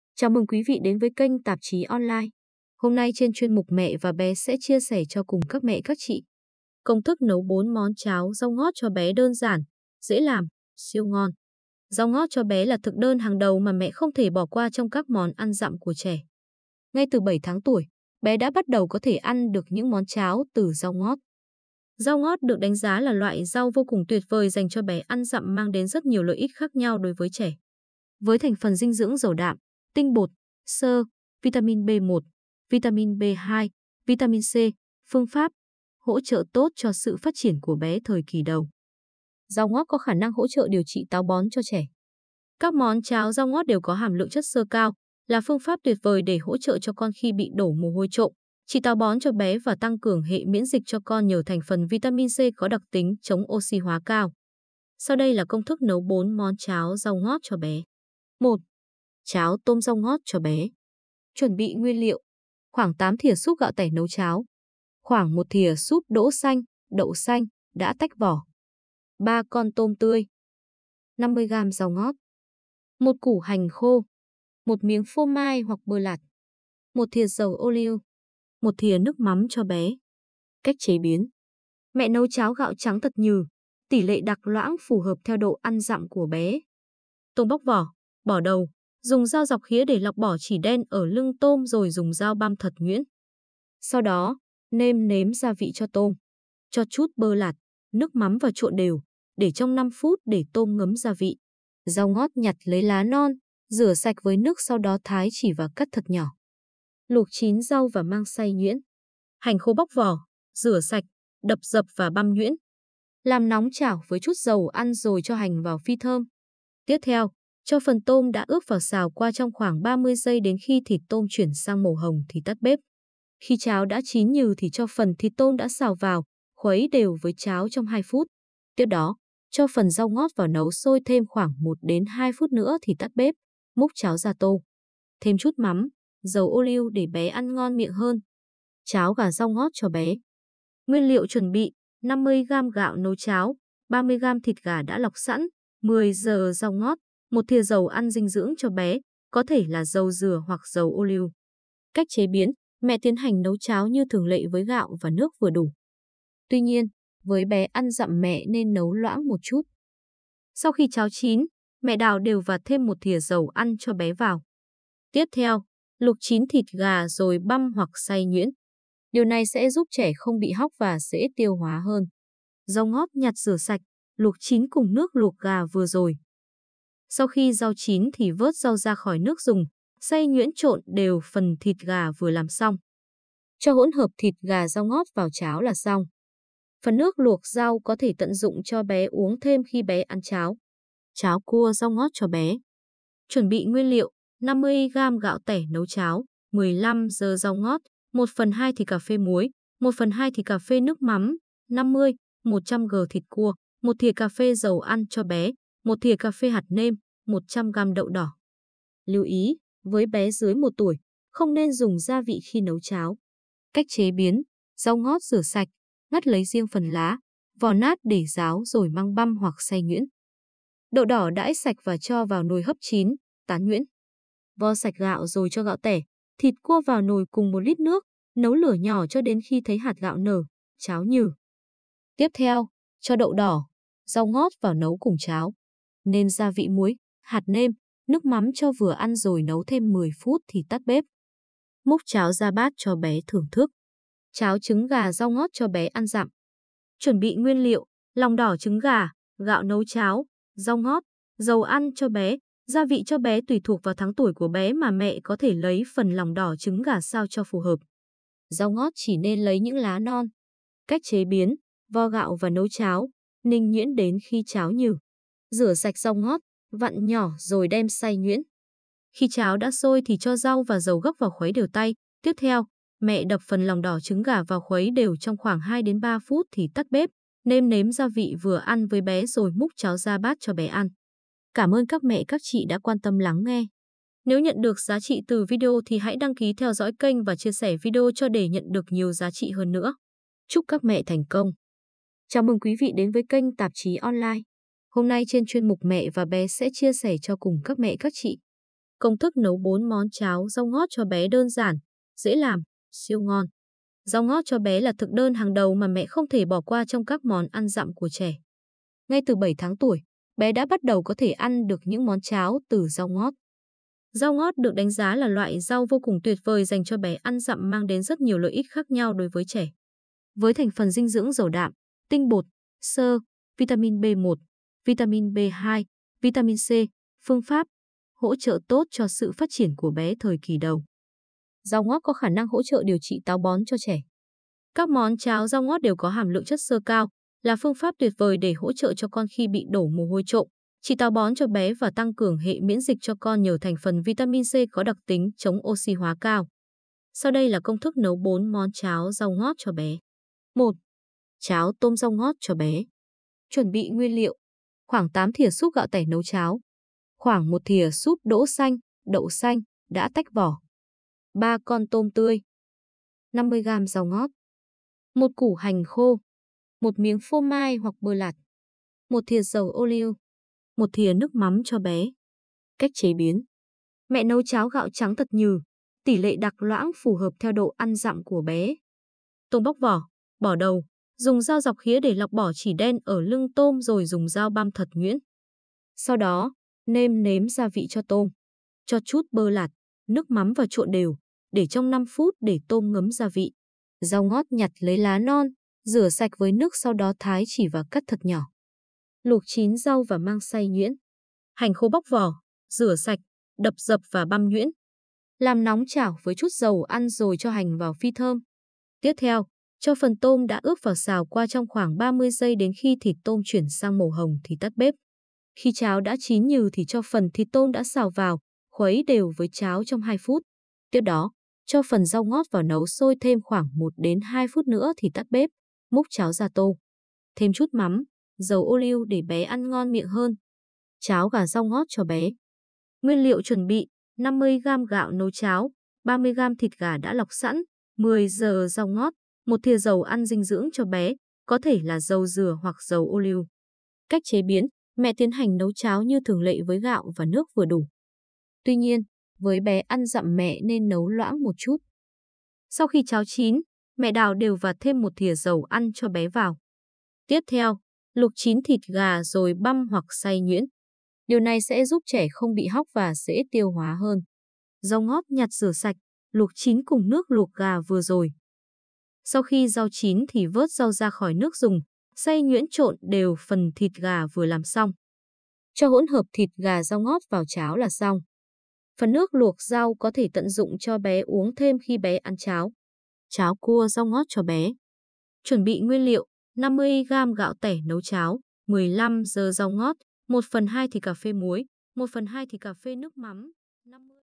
Công Thức Nấu Cháo Rau Ngót Cho Bé Siêu Ngon
Chào mừng quý vị đến với kênh tạp chí online. hôm nay trên chuyên mục mẹ và bé sẽ chia sẻ cho cùng các mẹ các chị công thức nấu 4 món cháo rau ngót cho bé đơn giản, dễ làm, siêu ngon Rau ngót cho bé là thực đơn hàng đầu mà mẹ không thể bỏ qua trong các món ăn dặm của trẻ. Ngay từ 7 tháng tuổi, bé đã bắt đầu có thể ăn được những món cháo từ rau ngót. Rau ngót được đánh giá là loại rau vô cùng tuyệt vời dành cho bé ăn dặm mang đến rất nhiều lợi ích khác nhau đối với trẻ. Với thành phần dinh dưỡng giàu đạm, tinh bột, xơ, vitamin B1, vitamin B2, vitamin C, PP...hỗ trợ tốt cho sự phát triển của bé thời kỳ đầu. Rau ngót có khả năng hỗ trợ điều trị táo bón cho trẻ. Các món cháo rau ngót đều có hàm lượng chất xơ cao, là phương pháp tuyệt vời để hỗ trợ cho con khi bị đổ mồ hôi trộm, trị táo bón cho bé và tăng cường hệ miễn dịch cho con nhờ thành phần vitamin C có đặc tính chống oxy hóa cao. Sau Đây là Công thức nấu 4 món cháo rau ngót cho bé
1. Cháo tôm rau ngót cho bé
Chuẩn bị nguyên liệu:
- Khoảng 8 thìa súp gạo tẻ nấu cháo
- Khoảng 1 thìa súp đỗ xanh (đậu xanh) đã tách vỏ
- 3 con tôm tươi - 50g rau ngót - 1 củ hành khô - 1 miếng phô mai hoặc bơ lạt - 1 thìa dầu oliu - 1 thìa nước mắm cho bé
Cách chế biến:
- Mẹ nấu cháo gạo trắng thật nhừ, tỉ lệ đặc loãng phù hợp theo độ ăn dặm của bé.
 - Tôm bóc vỏ, bỏ đầu, dùng dao dọc khía để lọc bỏ chỉ đen ở lưng tôm rồi dùng dao băm thật nhuyễn. Sau đó, nêm nếm gia vị cho tôm. Cho chút bơ lạt, nước mắm vào trộn đều, để trong 5 phút để tôm ngấm gia vị.
- Rau ngót nhặt lấy lá non, rửa sạch với nước sau đó thái chỉ và cắt thật nhỏ. Luộc chín rau và mang xay nhuyễn.
- Hành khô bóc vỏ, rửa sạch, đập dập và băm nhuyễn. Làm nóng chảo với chút dầu ăn rồi cho hành vào phi thơm. Tiếp theo, cho phần tôm đã ướp vào xào qua trong khoảng 30 giây đến khi thịt tôm chuyển sang màu hồng thì tắt bếp.
 - Khi cháo đã chín nhừ thì cho phần thịt tôm đã xào vào, khuấy đều với cháo trong 2 phút. Tiếp đó, cho phần rau ngót vào nấu sôi thêm khoảng 1-2 phút nữa thì tắt bếp, múc cháo ra tô.
 - Thêm chút mắm, dầu ô liu để bé ăn ngon miệng hơn.
2 Cháo gà rau ngót cho bé
Nguyên liệu chuẩn bị: - 50g gạo nấu cháo - 30g thịt gà đã lọc sẵn - 10g rau ngót - 1 thìa dầu ăn dinh dưỡng cho bé (có thể là dầu dừa hoặc dầu oliu...)
Cách chế biến:
- Mẹ tiến hành nấu cháo như thường lệ với gạo và nước vừa đủ. Tuy nhiên, với bé ăn dặm mẹ nên nấu loãng một chút. Sau khi cháo chín, mẹ đảo đều và thêm 1 thìa dầu ăn cho bé vào.
 - Tiếp theo, luộc chín thịt gà rồi băm hoặc xay nhuyễn. Điều này sẽ giúp trẻ không bị hóc và dễ tiêu hóa hơn.
- Rau ngót nhặt rửa sạch, luộc chín cùng nước luộc gà vừa rồi. Sau khi rau chín thì vớt rau ra khỏi nước dùng, xay nhuyễn trộn đều phần thịt gà vừa làm xong. - Cho hỗn hợp thịt gà rau ngót vào cháo là xong. Phần nước luộc rau có thể tận dụng cho bé uống thêm khi bé ăn cháo.
3 Cháo cua rau ngót cho bé
Chuẩn bị nguyên liệu: - 50g gạo tẻ nấu cháo - 15g rau ngót - 1/2 thìa cà phê muối - 1/2 thìa cà phê nước mắm - 50- 100g thịt cua - 1 thìa cà phê dầu ăn cho bé - 1 thìa cà phê hạt nêm - 100g đậu đỏ
Lưu ý: Với bé dưới 1 tuổi, không nên dùng gia vị khi nấu cháo.
Cách chế biến:
- Rau ngót rửa sạch, ngắt lấy riêng phần lá, vò nát để ráo rồi mang băm hoặc xay nhuyễn. Đậu đỏ đãi sạch và cho vào nồi hấp chín, tán nhuyễn.
- Vo sạch gạo rồi cho gạo tẻ, thịt cua vào nồi cùng 1 lít nước, nấu lửa nhỏ cho đến khi thấy hạt gạo nở, cháo nhừ.
- Tiếp theo, cho đậu đỏ, rau ngót vào nấu cùng cháo. Nêm gia vị muối, hạt nêm, nước mắm cho vừa ăn rồi nấu thêm 10 phút thì tắt bếp. Múc cháo ra bát cho bé thưởng thức.
4 Cháo trứng gà rau ngót cho bé ăn dặm
Chuẩn bị nguyên liệu: - Lòng đỏ trứng gà - Gạo nấu cháo - Rau ngót - Dầu ăn cho bé - Gia vị cho bé Tùy thuộc vào tháng tuổi của bé mà mẹ có thể lấy phần lòng đỏ trứng gà sao cho phù hợp. Rau ngót chỉ nên lấy những lá non.
Cách chế biến: - Vo gạo và nấu cháo, ninh nhuyễn đến khi cháo nhừ. - Rửa sạch rau ngót, vặn nhỏ rồi đem xay nhuyễn.
 - Khi cháo đã sôi thì cho rau và dầu gấc vào khuấy đều tay - Tiếp theo, mẹ đập phần lòng đỏ trứng gà vào khuấy đều trong khoảng 2-3 phút thì tắt bếp, nêm nếm gia vị vừa ăn với bé rồi múc cháo ra bát cho bé ăn.
Cảm ơn các mẹ các chị đã quan tâm lắng nghe. nếu nhận được giá trị từ video thì hãy đăng ký theo dõi kênh và chia sẻ video cho để nhận được nhiều giá trị hơn nữa.
Chúc các mẹ thành công